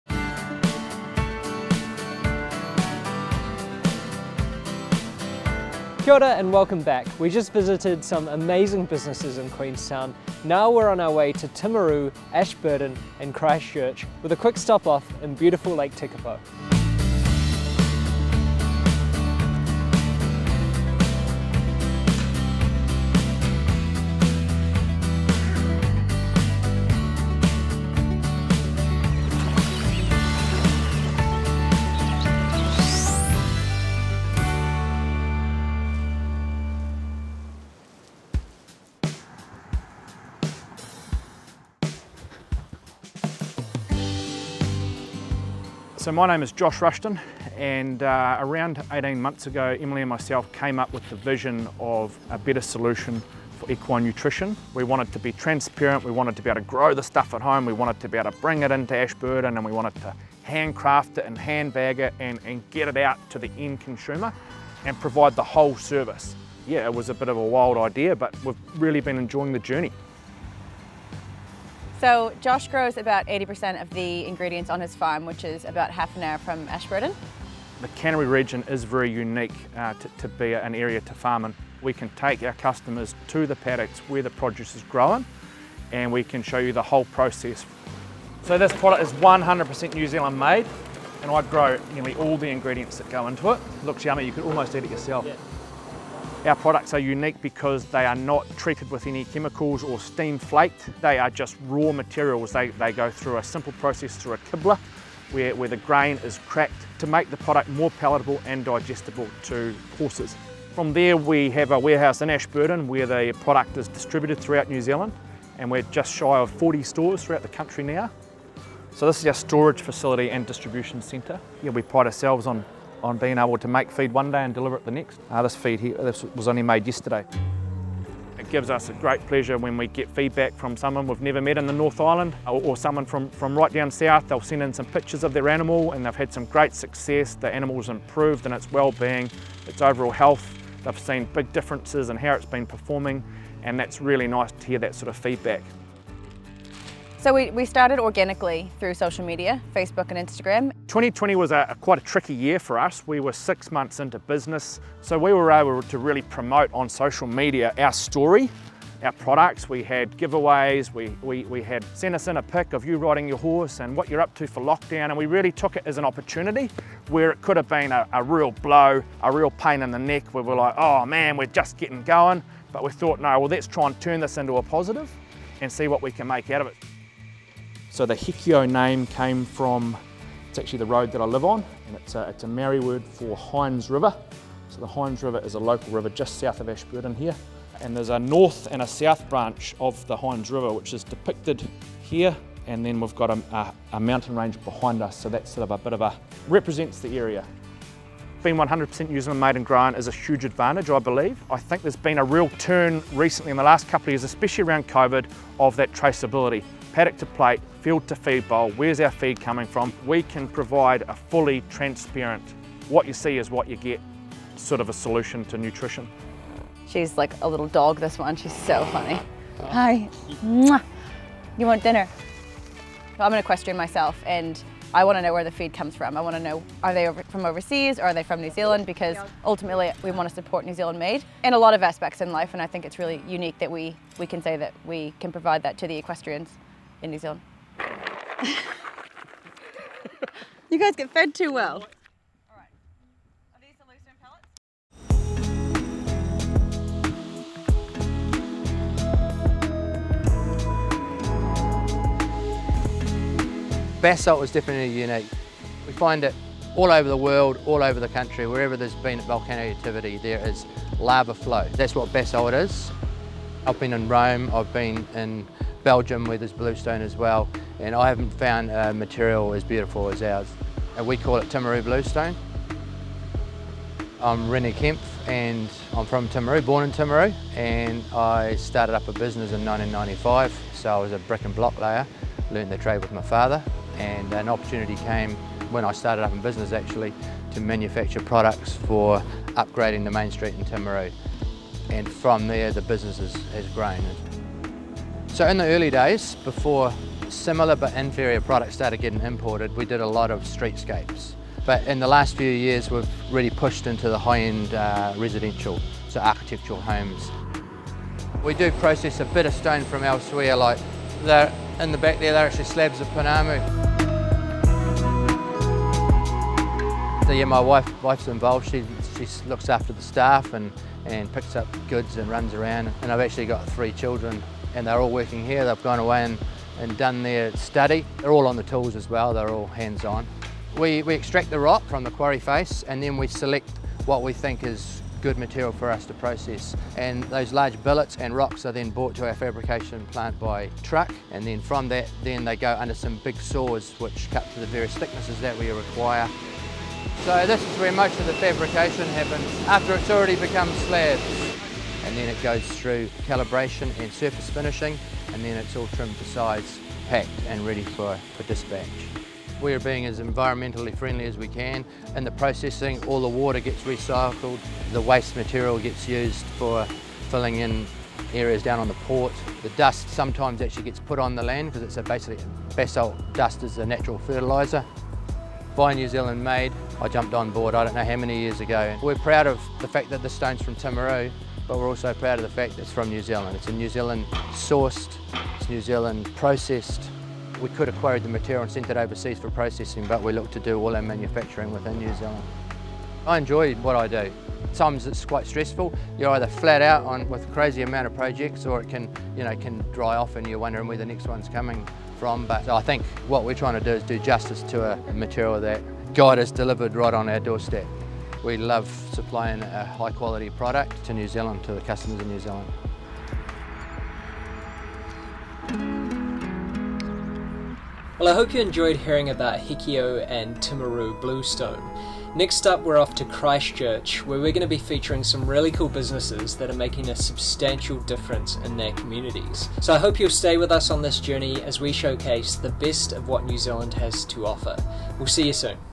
A Kiwi Original | Canterbury | Episode 2
We're headed North! The A Kiwi Original trip across Te Waipounamu (South Island) takes us to Timaru and Ashburton, where we meet up with Timaru Bluestone and Hekeao Feed Co.

Hekeao Feed Co. is a new business that is making waves in the equine nutrition space. Most of the ingredients are grown on the Director, Josh Rushton's farm. Hekeao utilises the unique agricultural landscape of Ashburton to grow nutritious and delicious feed that you can trace from farm to feed-bowl.

Timaru Bluestone is a family business that again utilises the natural advantage of the Canterbury region. Forged in lava flow of ancient volcanoes, the bluestone that surrounds the Timaru area makes for decadent and beautiful material in architectural homes.

A KIWI ORIGINAL
Watch uniquely Kiwi stories from people excelling across our many varied regions.

TIMESTAMPS
0:36 Hekeao Feed Co.
4:10 Timaru Bluestone

GET FEATURED ON A KIWI ORIGINAL

MAIL
3-11 Hunter Street | Wellington | New Zealand.